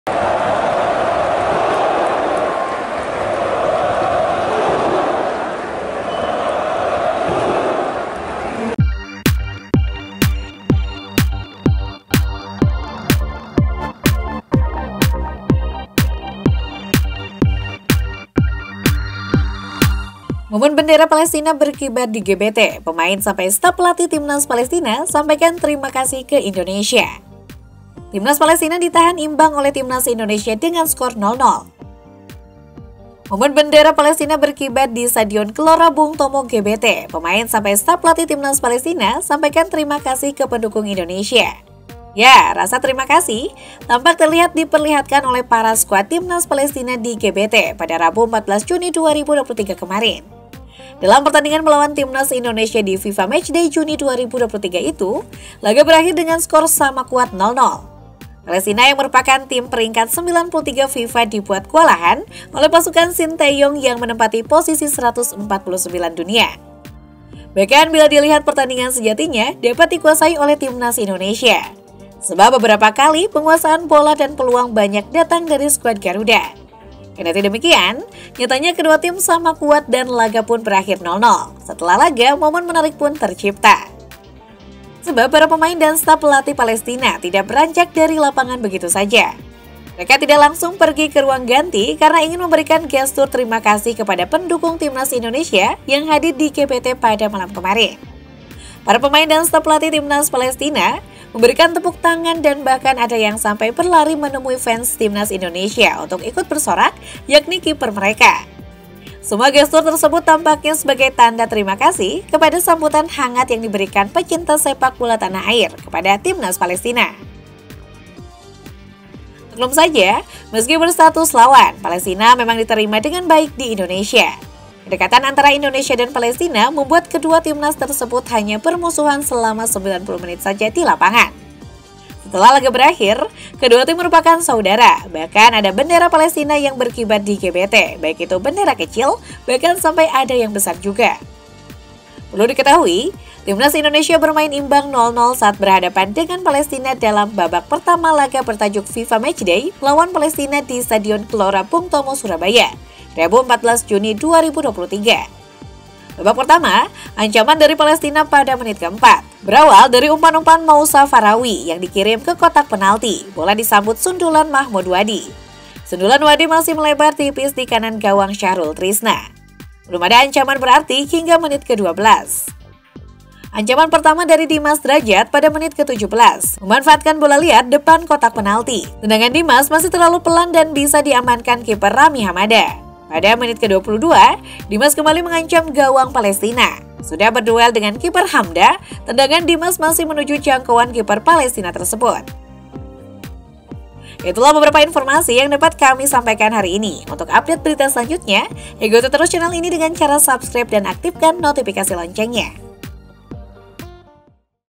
BIKIN TERHARU ⚡ MOMEN TIMNAS PALESTINA BERI HORMAT TIMNAS INDONESIA DAN SUPORTER INDONESIA 😍
Momen Bendera Palestina Berkibar di GBT, Pemain Sampai Staf Pelatih Timnas Palestina Sampaikan Terima Kasih ke Indonesia.
Timnas Palestina di tahan imbang Oleh timnas Indonesia dengan skor 0-0.

MOMEN bendera Palestina berkibar di Stadion Gelora Bung Tomo (GBT), pemain sampai staf pelatih Tmnas Palestina sampaikan terima kasih ke pendukung Indonesia. Ya, rasa terima kasih tampak terlihat diperlihatkan oleh para skuad Timnas Palestina di GBT pada Rabu 14 Juni 2023 kemarin.
Dalam pertandingan melawan Timnas Indonesia di FIFA Matchday Juni 2023 itu, laga berakhir dengan skor sama kuat 0-0. Palestina yang merupakan tim peringkat 93 FIFA dibuat kewalahan oleh pasukan Shin Tae-yong yang menempati posisi 149 dunia.
Bahkan bila dilihat pertandingan sejatinya dapat dikuasai oleh Timnas Indonesia. Sebab beberapa kali penguasaan bola dan peluang banyak datang dari skuad Garuda.
Kendati demikian, nyatanya kedua tim sama kuat dan laga pun berakhir 0-0. Setelah laga, momen menarik pun tercipta.